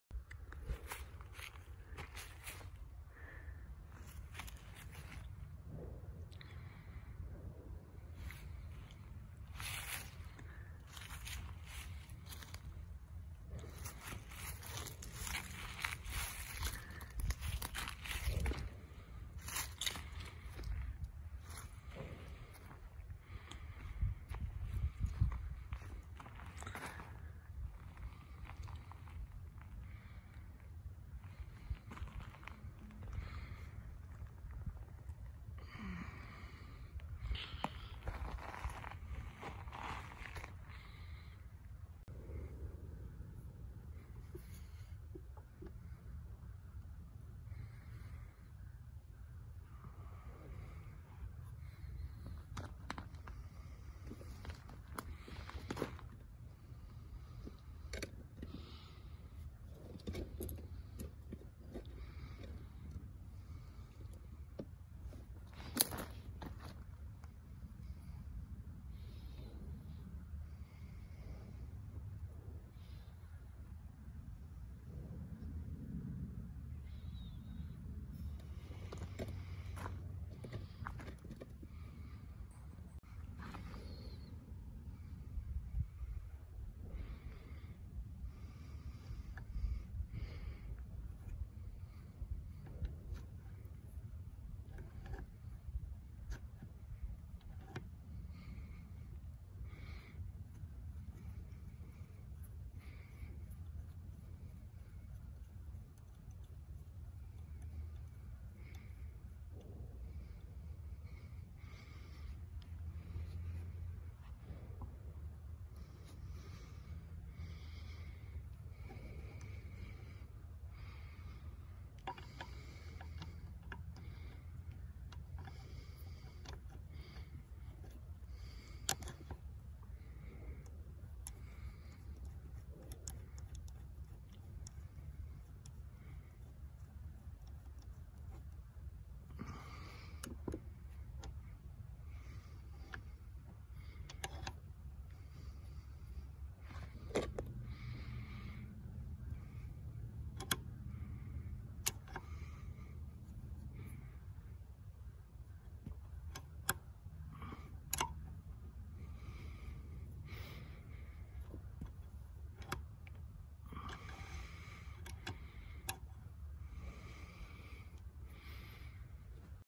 Naval Late 19th Century Deck Gun features
This is a model of a late 19th century breech loading naval gun in 50 caliber.